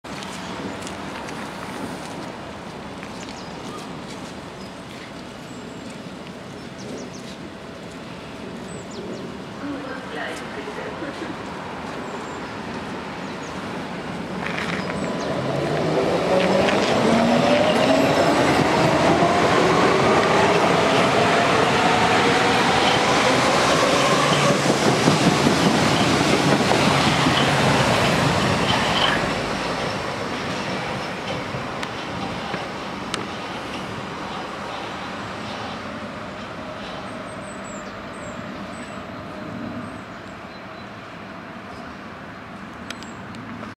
U1 nach Ohlstedt in Volksdorf
Während ich nun in Volksdorf auf die Rükkehr des DT5 wartete, fuhr mir noch ein DT3 vor die Nase. Dieser war gerade als 3-teilige Einheit (DT3 + DT3 + DT3-E) auf dem Weg nach Ohlstedt